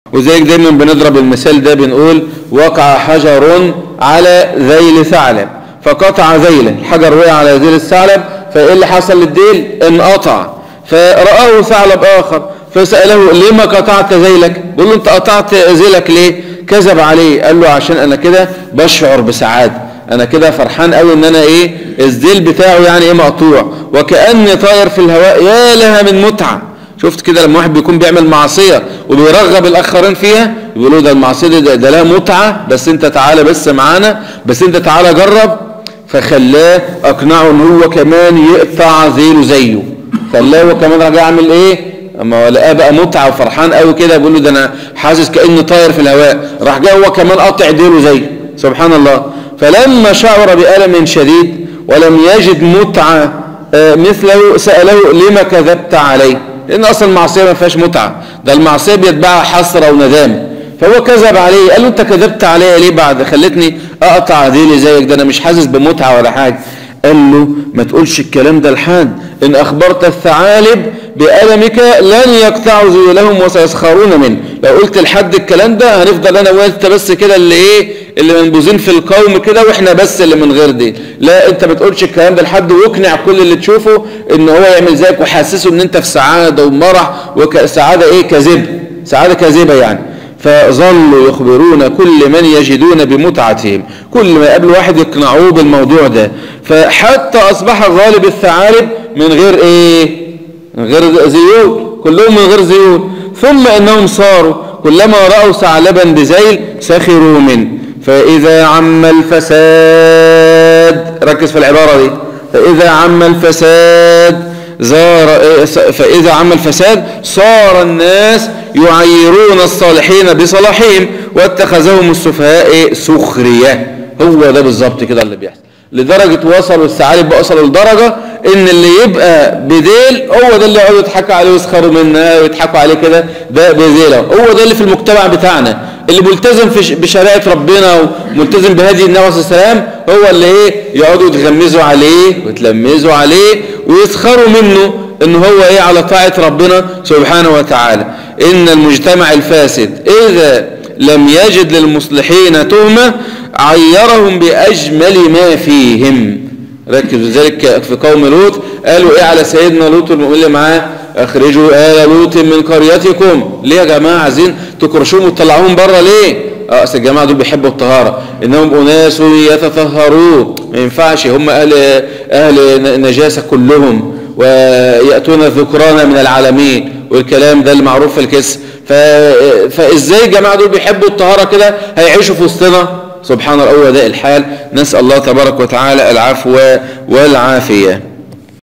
هل تعلم قصة الثعلب الذى قطع زيله وماذا حدث لبقية الثعالب(قصة معبرة جدا للواقع)
لـكل من يرغب في حفظ القرآن ،،،التواصل عبر واتساب ، هاتف رقم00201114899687